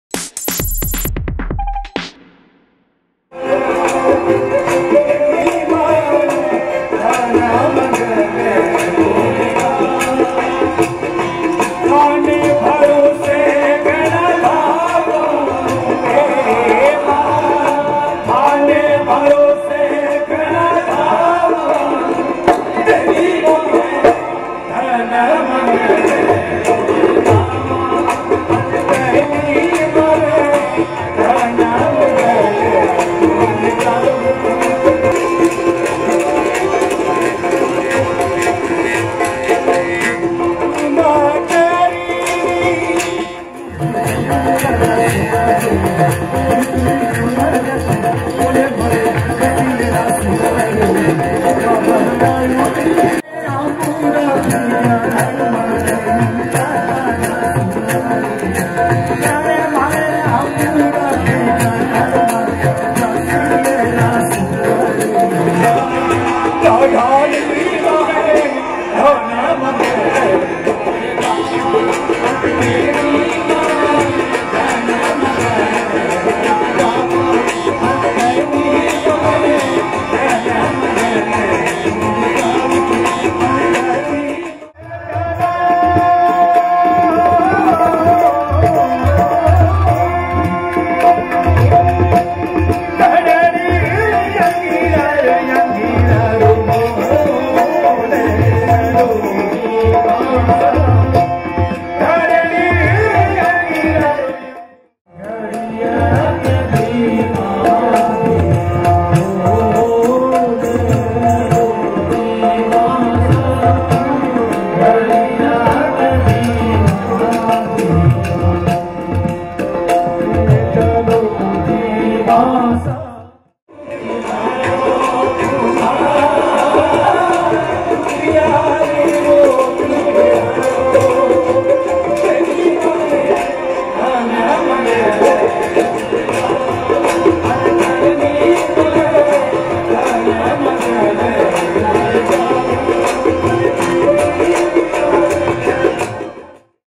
Lok Rang, canciones tradicionales que hablan de la vida
Uno de los prepresnates de la riqueza cultural en el arte clásico y tradicional de Rajastán, India es Samandar Khan, quien presenta Lok Rang con melodías manganiar acompañado de bailes y sintonias.